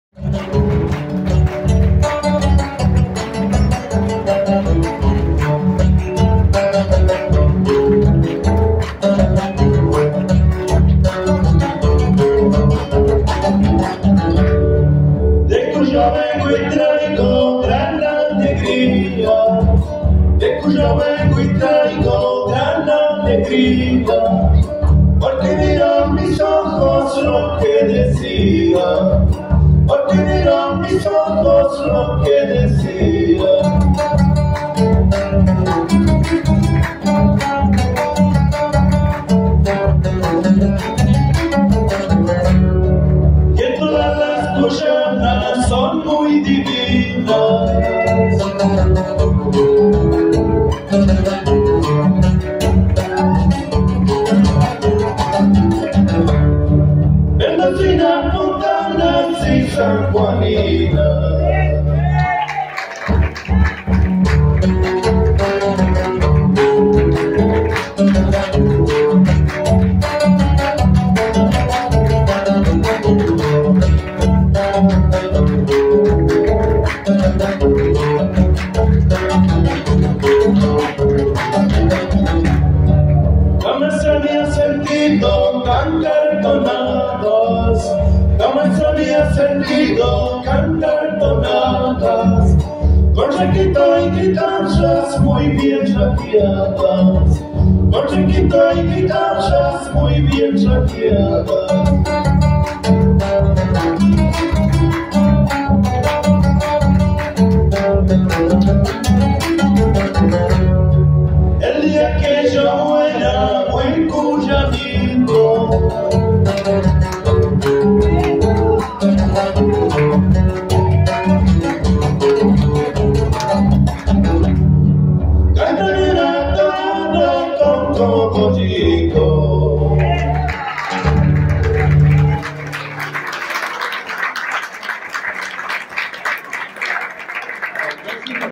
DE CUYO VENGO. SABORES CUYANOS
GATO DE FELIX BLANCO
HOMENAJE A RAUL TRANSITO AVILA (SAPO)
BOLICHE DON MIRANDA VILLA MERCEDES SAN LUIS